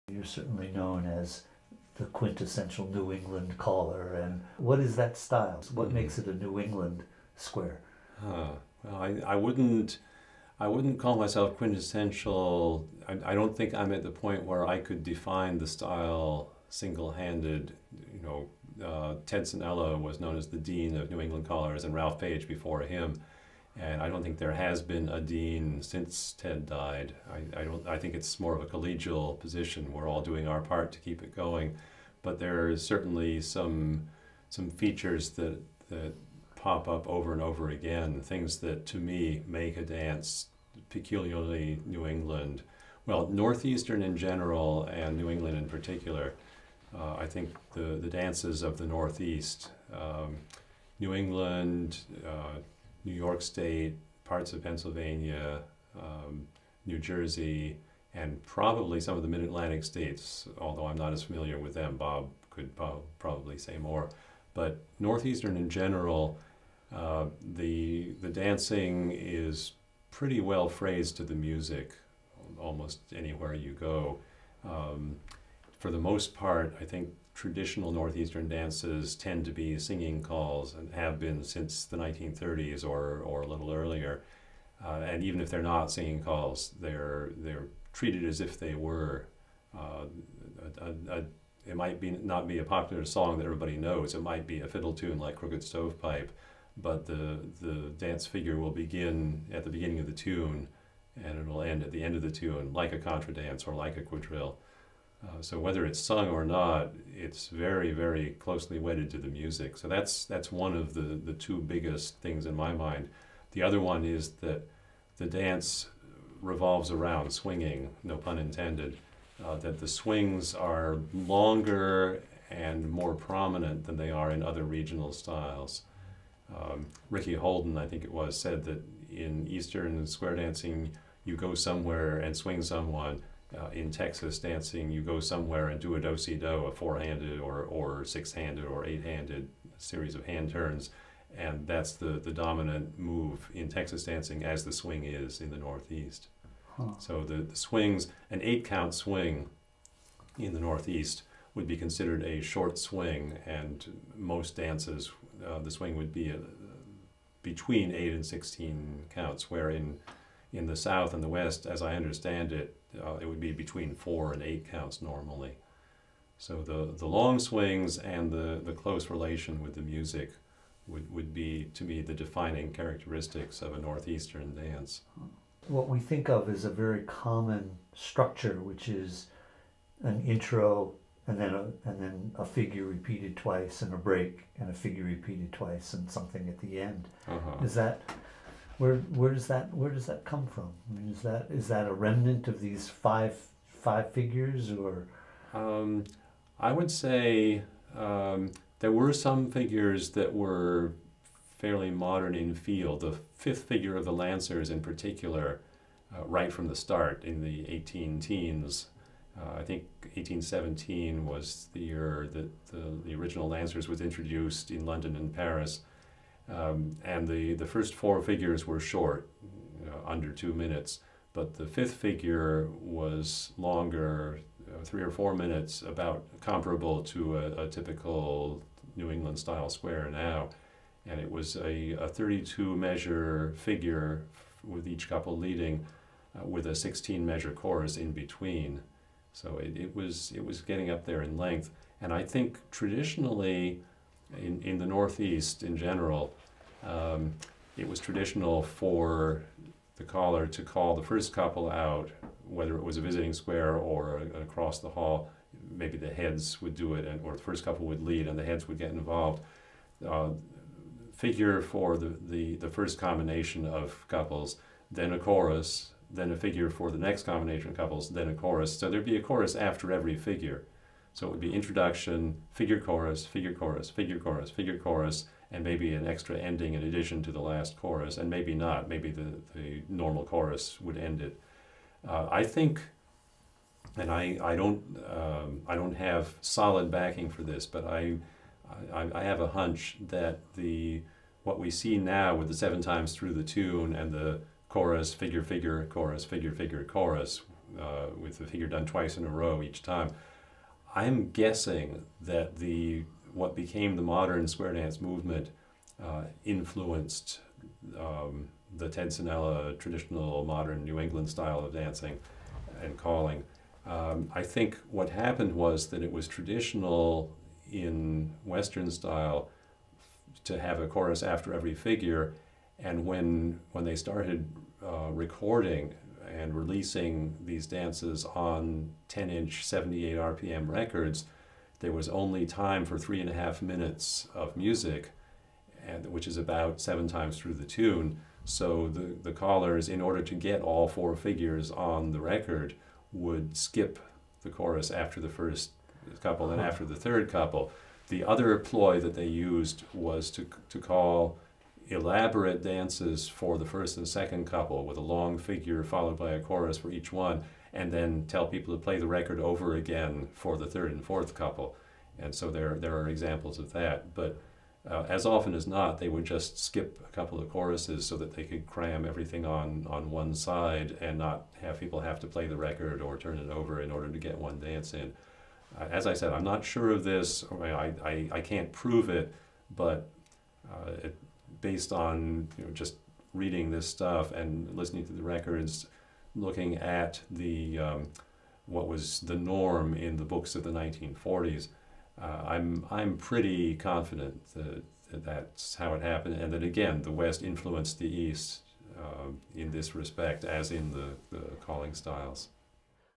Tony Parkes 1 - New England squares
Caller Tony Parkes discusses the typical characteristics of northeastern traditional squares in general, and New England squares in particular.

Recorded November 18, 2011, at the Dare To Be Square dance weekend, John C. Campbell Folk School, Brasstown, NC. Interview conducted by David Millstone and videotaped by John-Michael Seng-Wheeler, with financial support provided by Country Dance and Song Society. This documentation is part of the Square Dance History Project. Dance videos from the weekend: